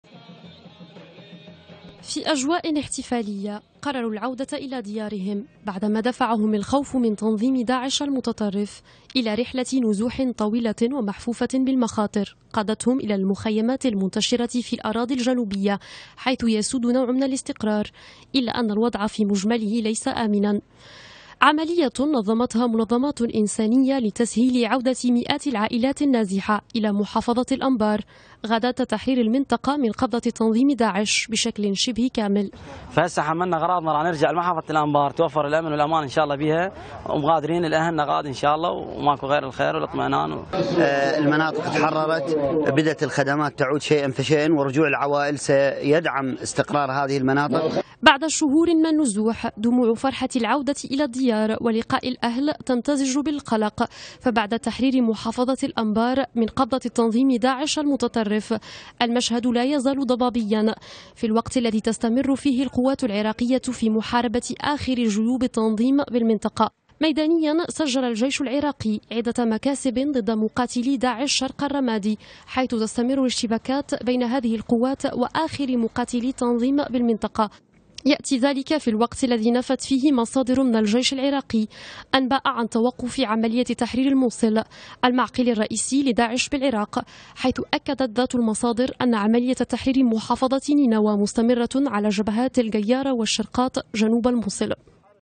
آمال عريضة في عودة الاستقرار لمحافظة الأنبار
بعد تحرير الجيش العراقي شبه الكامل لمحافظة الأنبار، بدأ المئاتُ من النازحين بالعودة إلى مدنهم الأصلية.

أعداد كبيرة منهم تأمل في عودة الأمن والأمان .